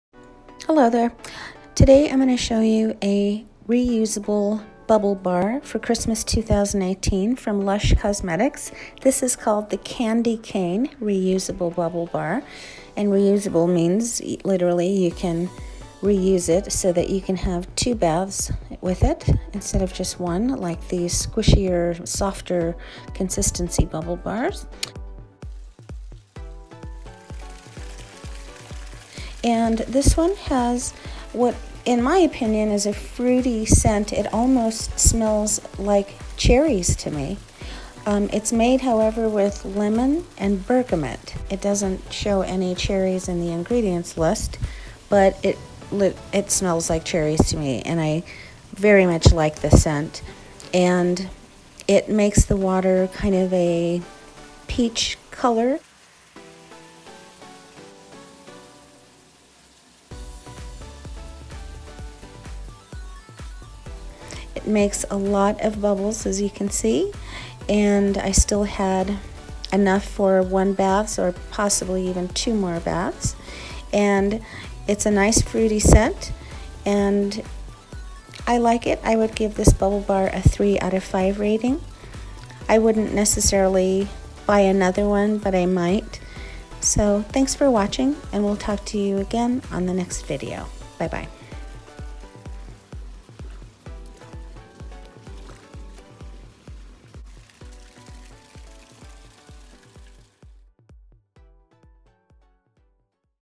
Lush 'Candy Cane' bubble bar
3 out of 5 rating.
Lush says its a new scent.
Reusable bubble bar made with lemon and bergamot, but for some reason it smells kind of like cherries to me.